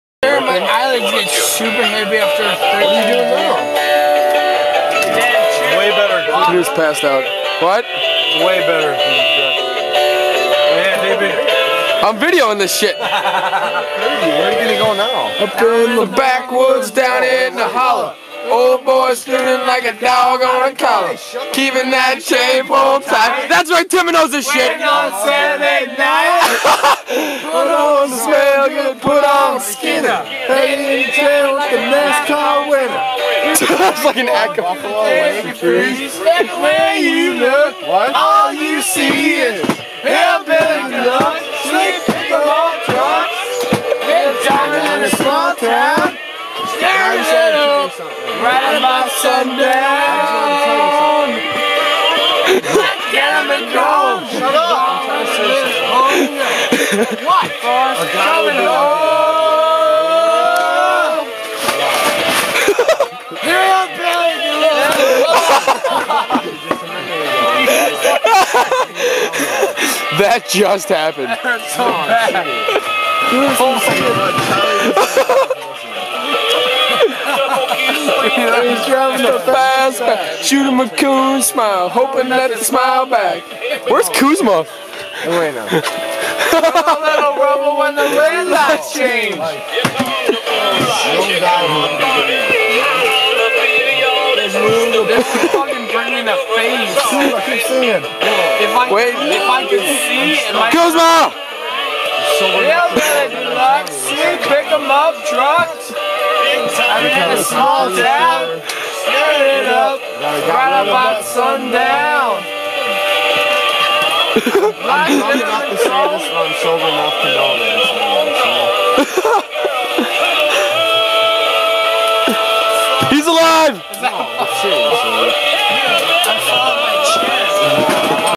The Ice Fishing Video
What do you get when you put 7 drunk Rednecks on lake Buffaluffalo in the Great State of Minnesota.